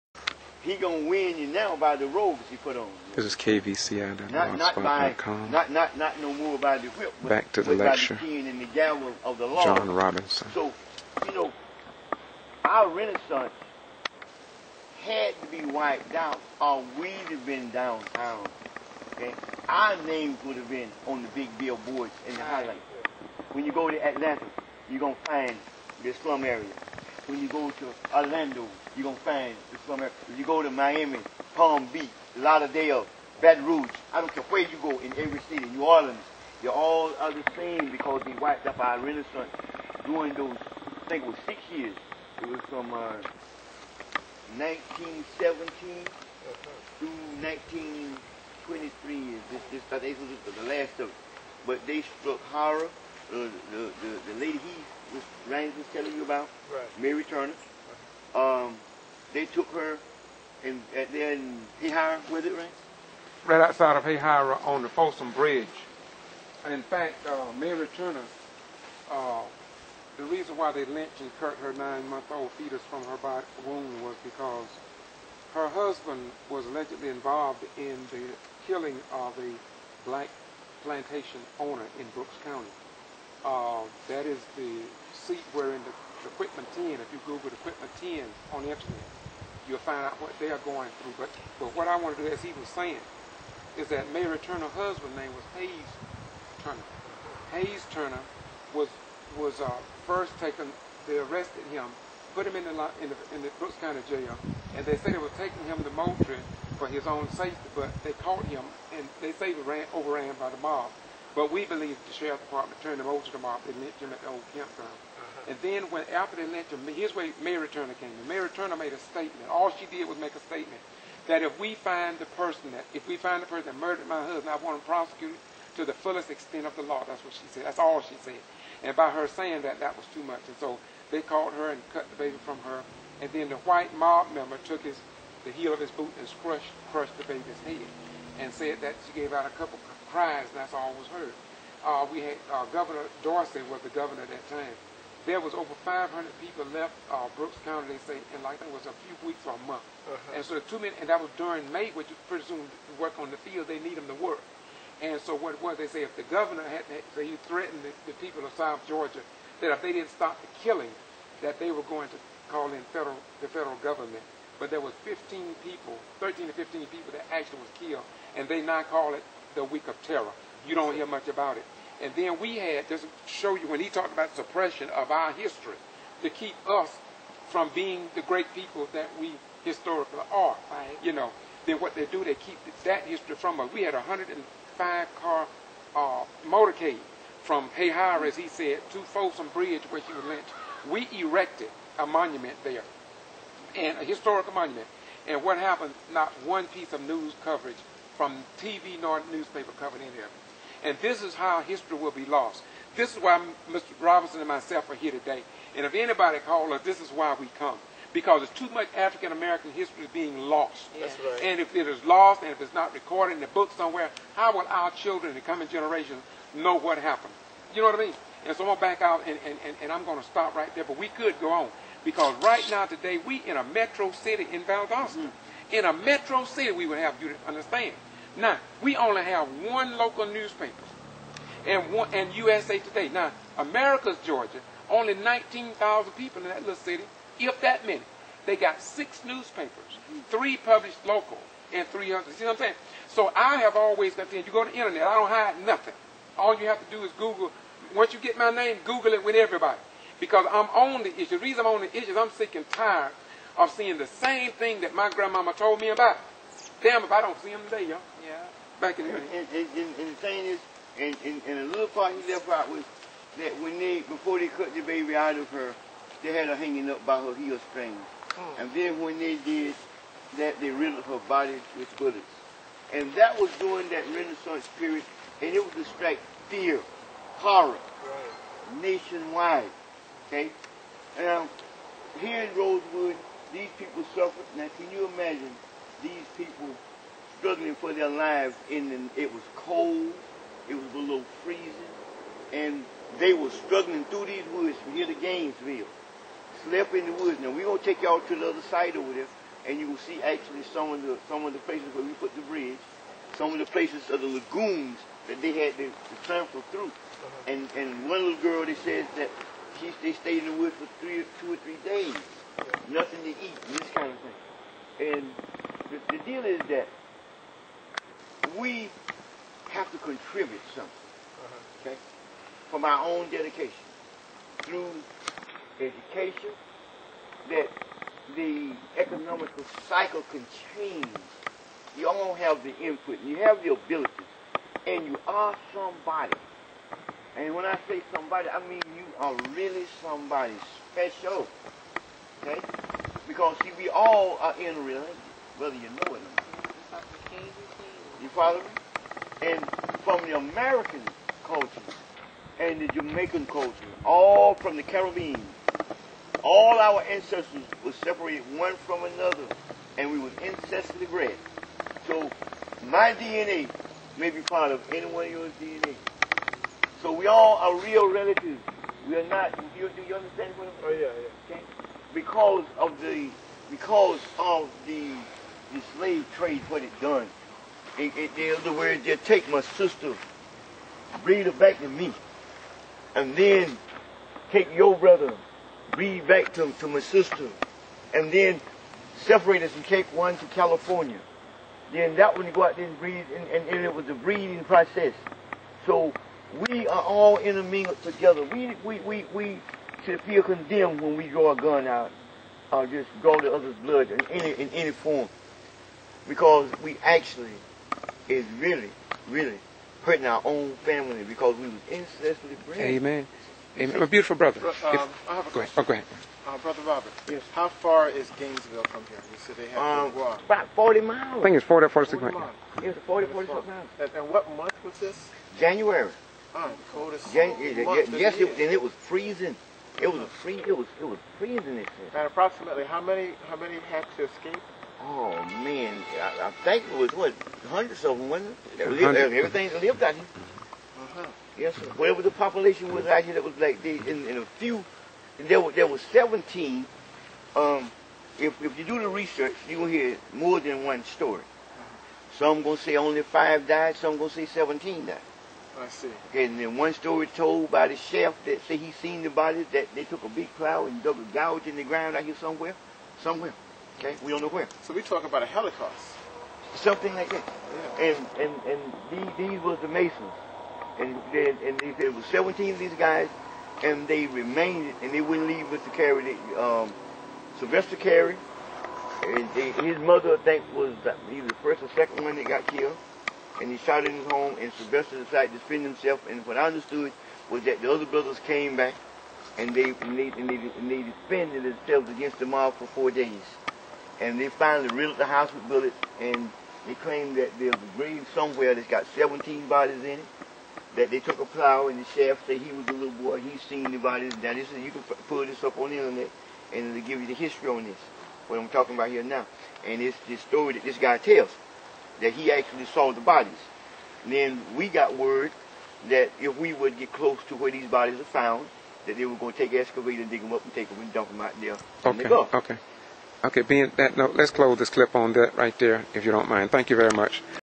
#5. Rosewood, Florida, John Robinson, George Boston Rhynes (At Old Prince Hall Masons)
(Updated link across the nation and work still being done in other places)

5. Valdosta NOT diverse in displaying pictures and accomplishments of all Citizens in our beloved city

12. Rhynes in Bentonville, Arkansas and 141 people were almost arrested.  This is Wal-Mart and shows how little they care about workers

16. Rhynes in Rosewood, Florida but with all the work we have done no mention is made about our work in Valdosta-Lowndes County Georgia.